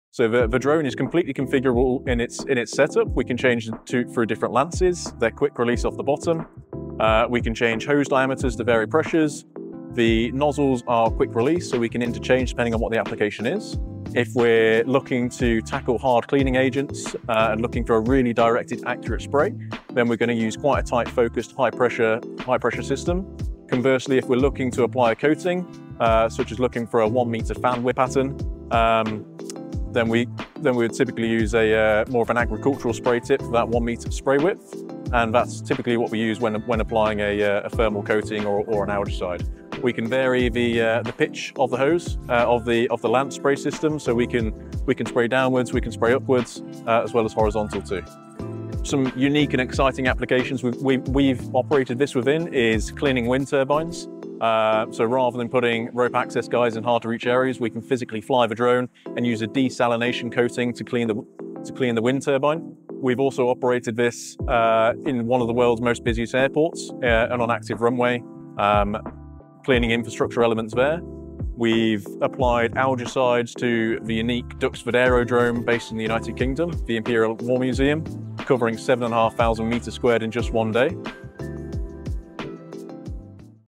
🌟 Introducing the Wash Drones AC4 🌟
The Wash Drone AC4 is the pinnacle of innovation and versatility in drone cleaning technology. Designed for seamless operation across diverse applications, the AC4 is the #1 choice for Wash Drones® accredited operators in 2025. Its state-of-the-art features and unmatched reliability make it a game-changer for industries worldwide.

✅ Key Benefits:

Precision cleaning for hard-to-reach surfaces
Enhanced safety and efficiency
Ideal for solar panels, facades, maritime applications, and more
Backed by the expertise of Wash Drones®
Now available via the Wash Drones® Store for USD $33,000.
Ready to transform your operations? Contact Wash Drones® to get started!